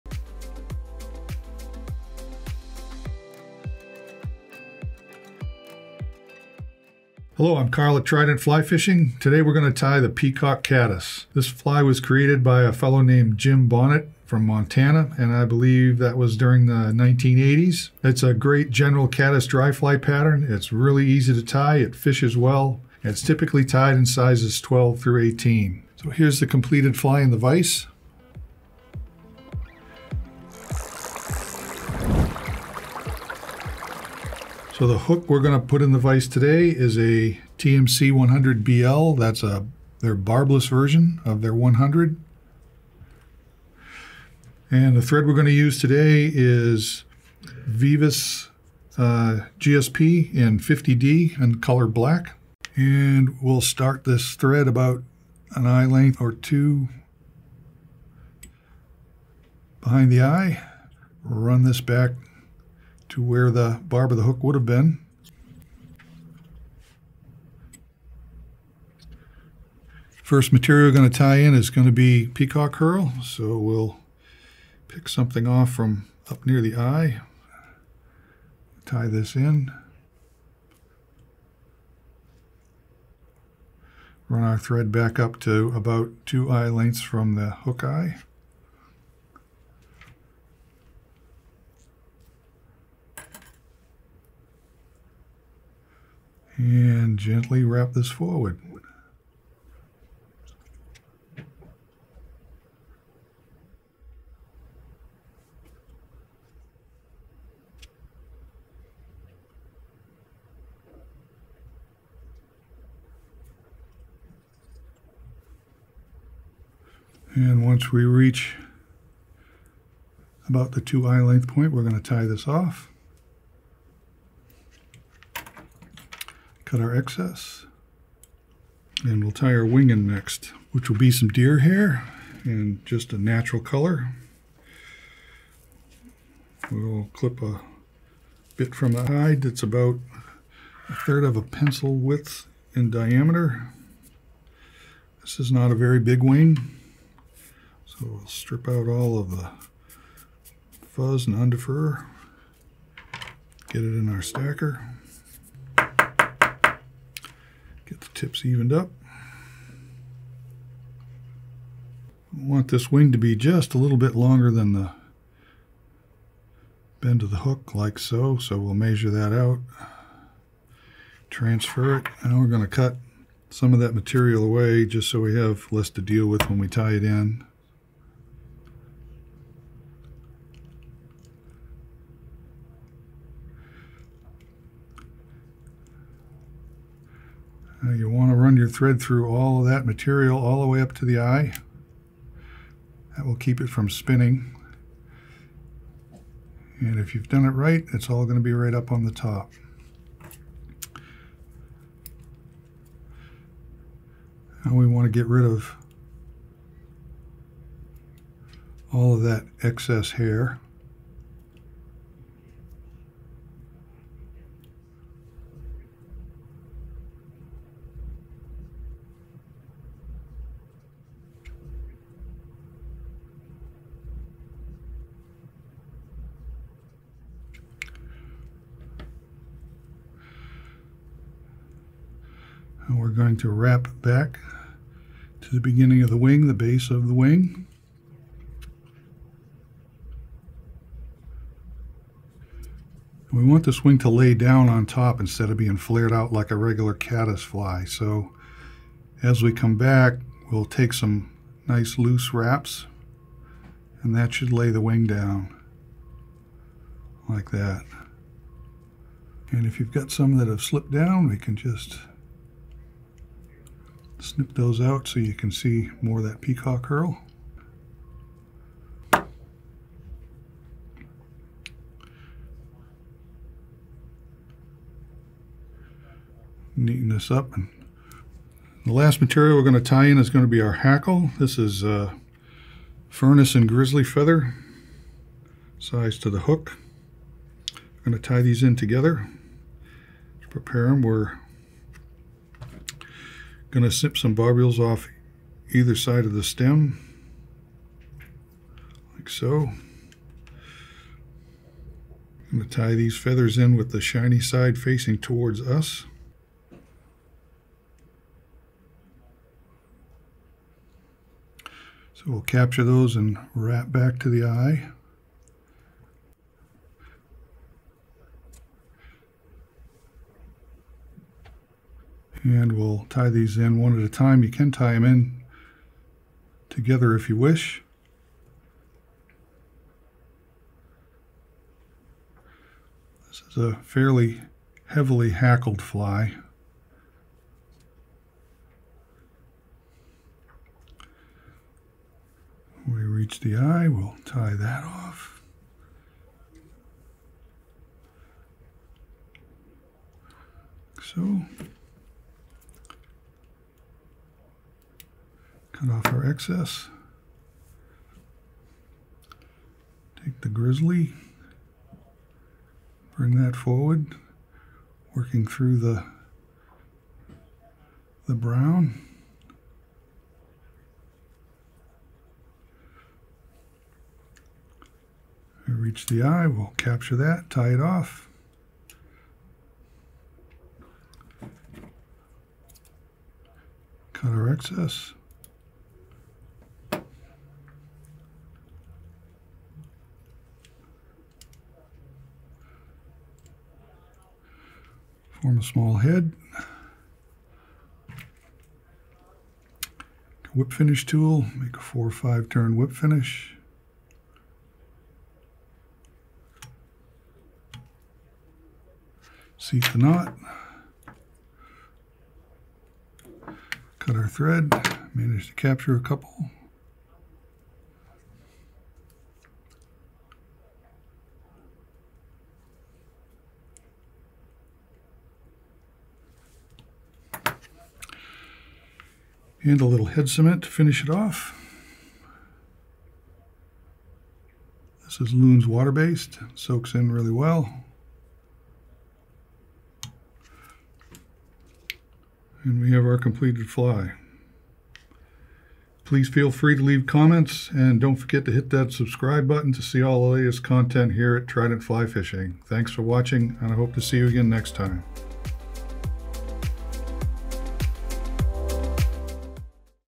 Peacock Caddis Dry Fly | A Simple Trout Fly | Fly Tying Tutorial
This fly was created by a fellow named Jim Bonnett from Montana I believe during the 1980s. It’s a great general caddis dry fly pattern. It fishes really well and it's easy and quick to tie. It’s typically tied in sizes 12 -18.

Chapters
0:00 - Intro
0:35 - Hook And Thread
1:10 - Peacock Herl
2:10 - Deer Hair
4:40 - Feather

Got Questions?